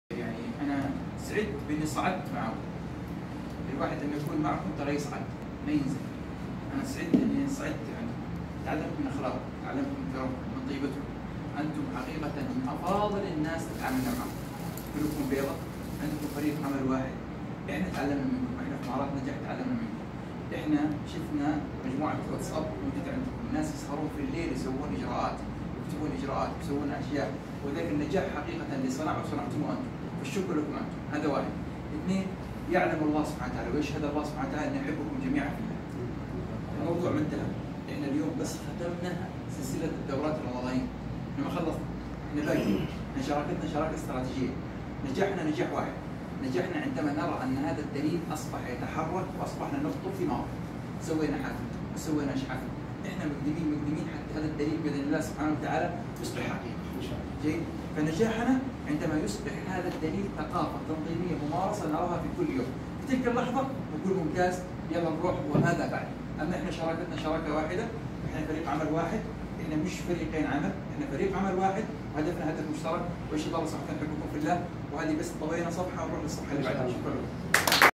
من فعاليات دورة مهارات العرض والتقديم مع المدرب د. محمد العامري
يطيب لنا في مهارات النجاح أن نقدم لكم مقتطفات من فعاليات دورة مهارات العرض والتقديم
Presentation and presentation skills course

و التي قدمها المدرب والمستشار التعليمي الدكتور محمد العامري
للقيادات الإدارية في شركة مطاعم قصر إريز في الرياض في المملكة العربية السعودية

والله الموفق